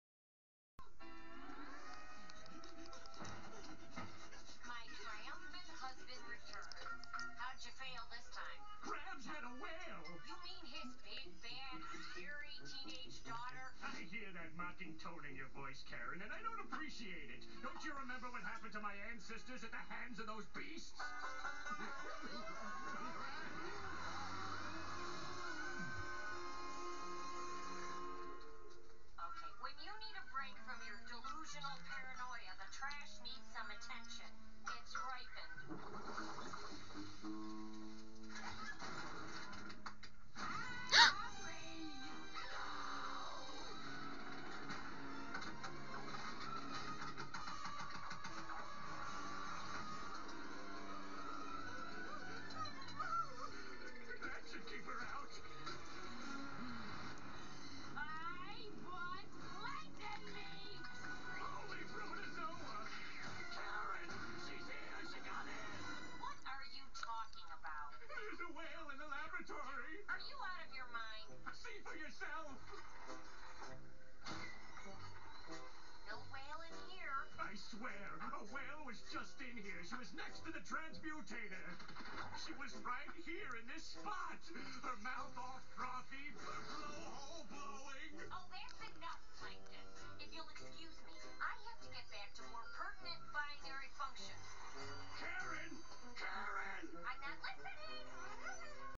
He Recalls The Time When His Ancestors Were Eaten 😮😮
Plankton sarcastically tells Karen that his ancestors were eaten by a whale and he cannot deal with this crap at all, Karen is not having that